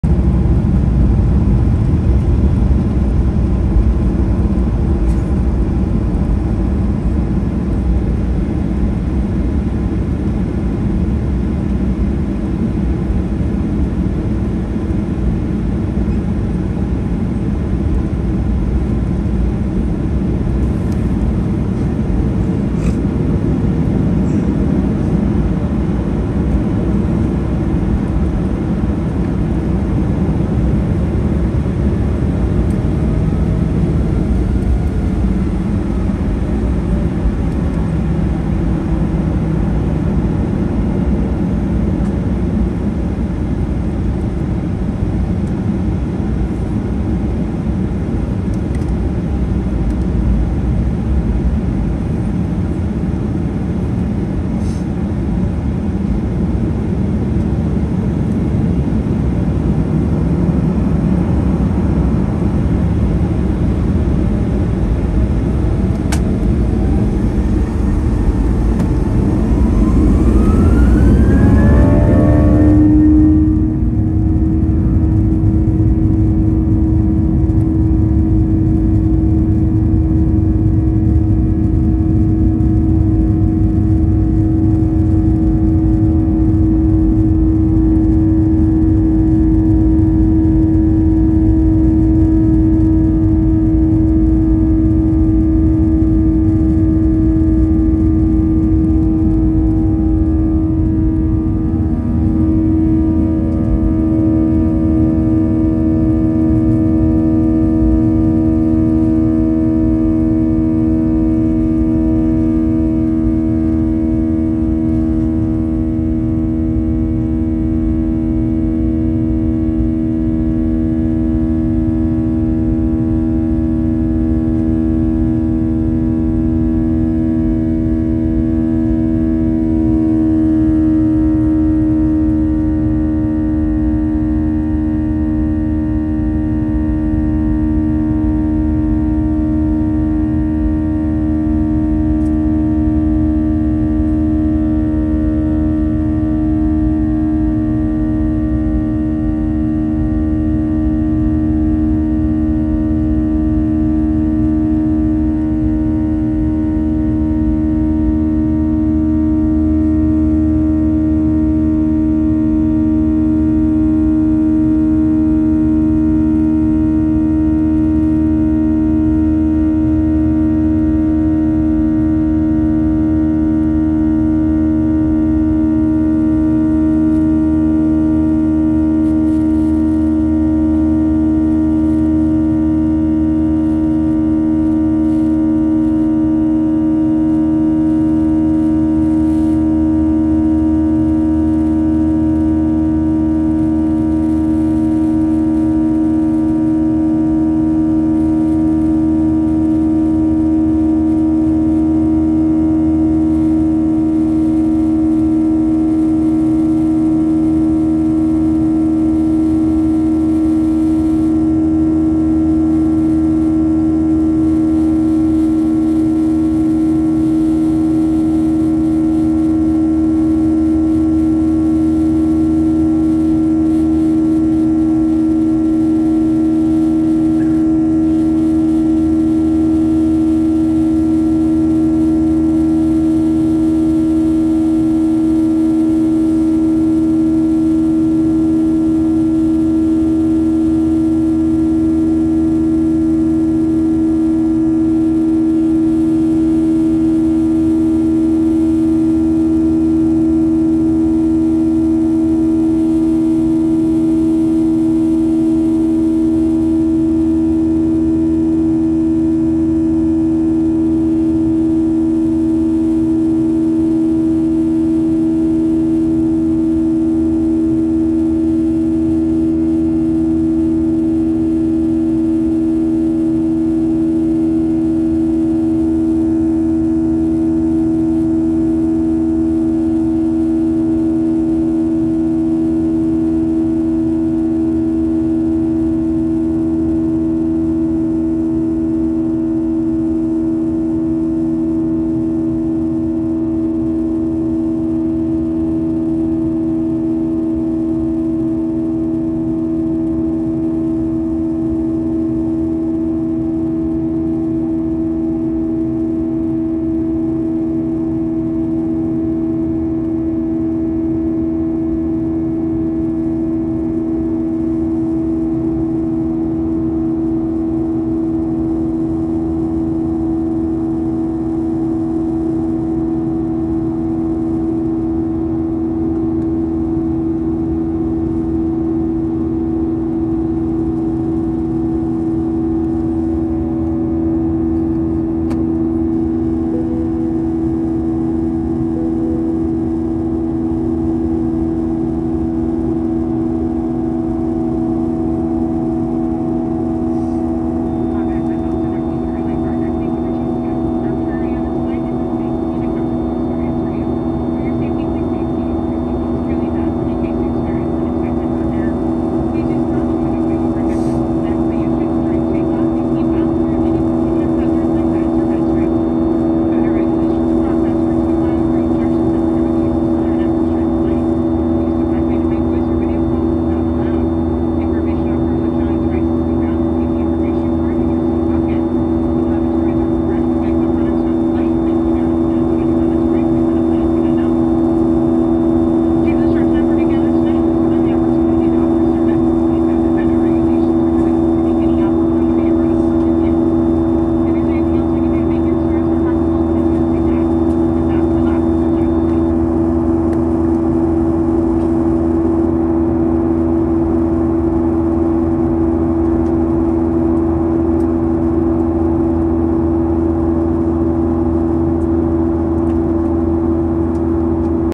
Delta Air Lines Boeing 737-900ER (Winglets) Takeoff from Seattle-Tacoma International Airport
Date: January 15, 2024
Tail Number: N817DN
Flight Number: DL2013
Origin: Seattle-Tacoma International Airport (SEA)
Destination: Spokane International Airport (GEG)
Seat: 5A
Cabin: First Class
Runway: 16L
Departure Gate: A8